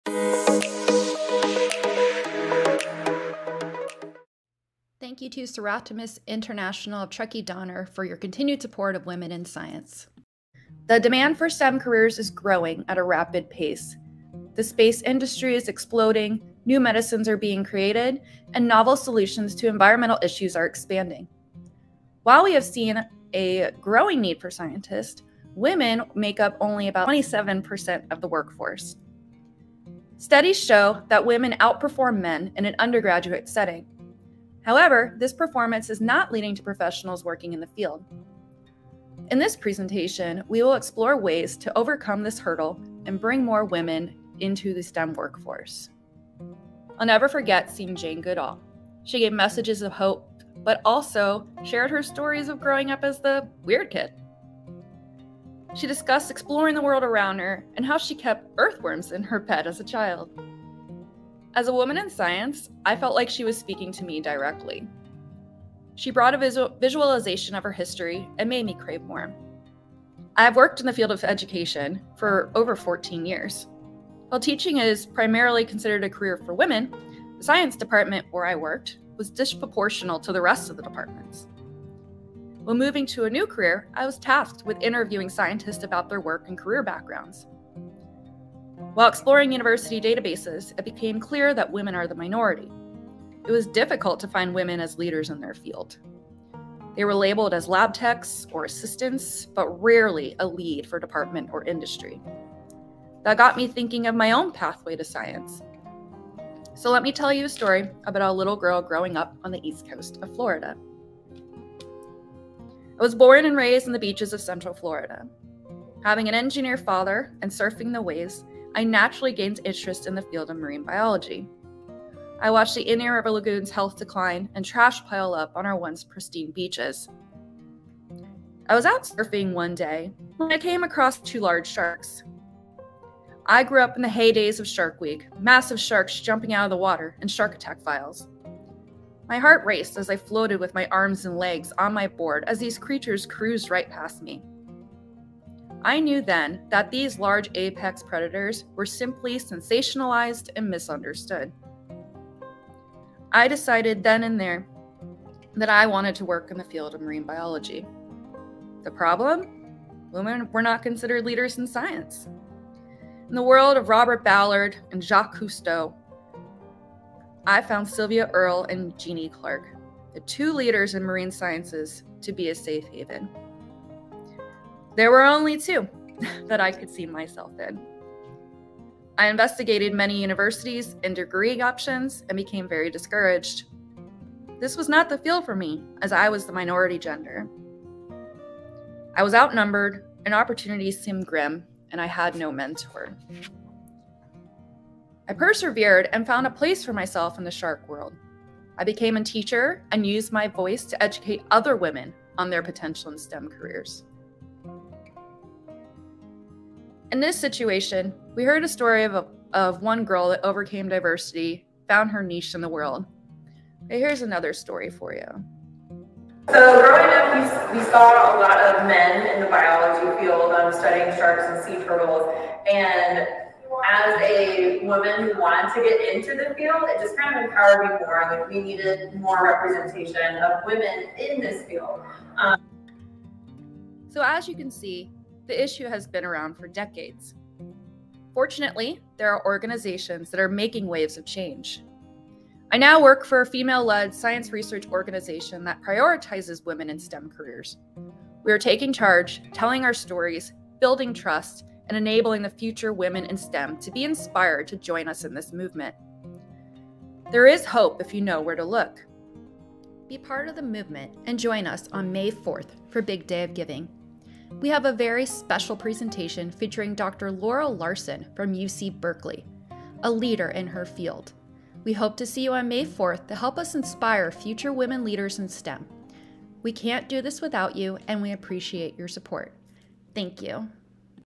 Women in STEM
Join for us a presentation by our Program and Outreach Manager, Jenn Cotton. In this video she shares her story of growth as a woman in STEM and provides insight to how YOU can get involved! She shares her story of working in the field of marinescience and sharks. teaching was her primary career path but the ocean got her there.